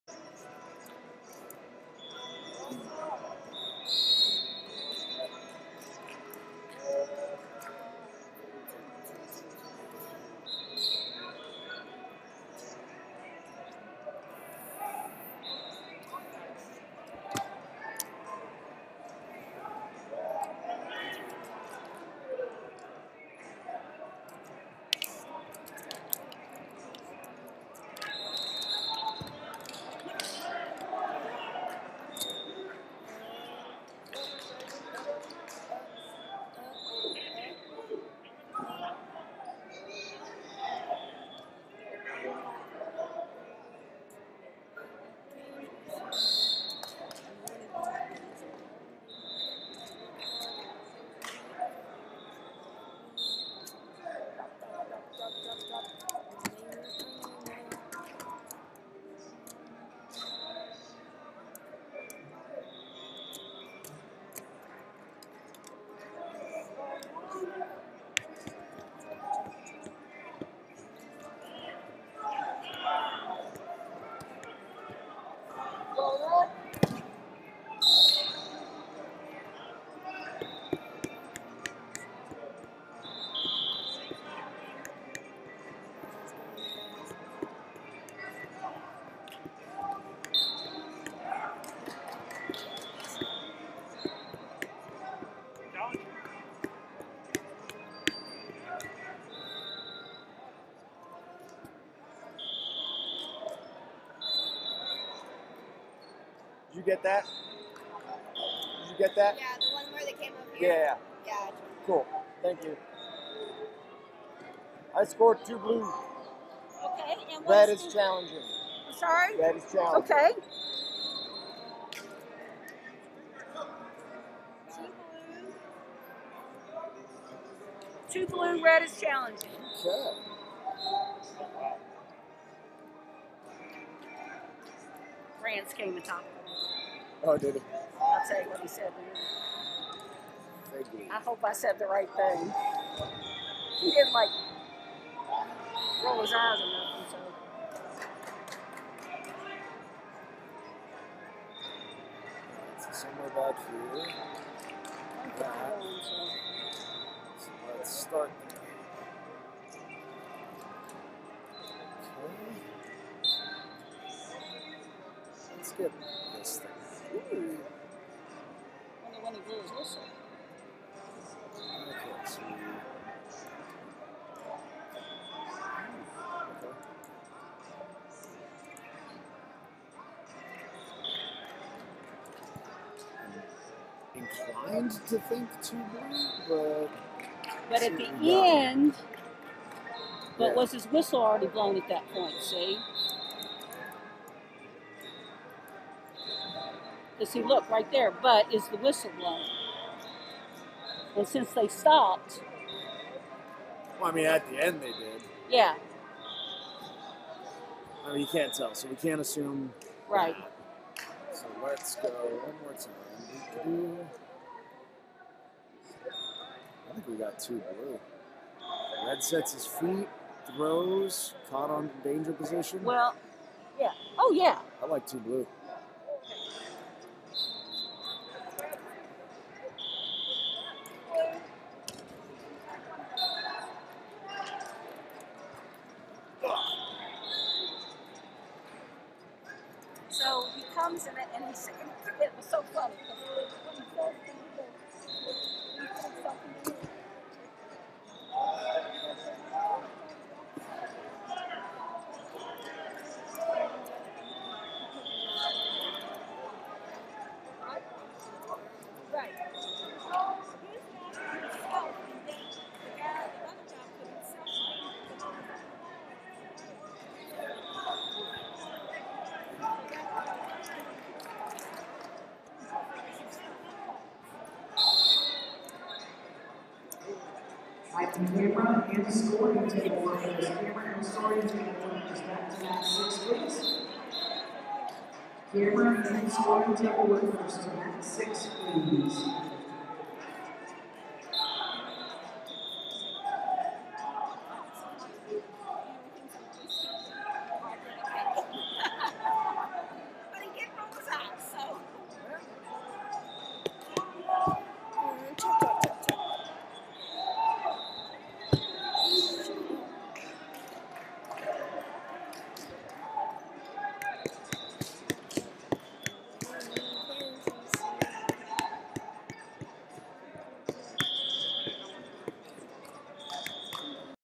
86 Kg Prelims Colin Fegley Unattached Vs Kenneth Prince Nebraska Golden Eagles Wrestling Club
2020 UWW Jr And U23 Nationals - 86 Kg Prelims Colin Fegley Unattached Vs Kenneth Prince Nebraska Golden Eagles Wrestling Club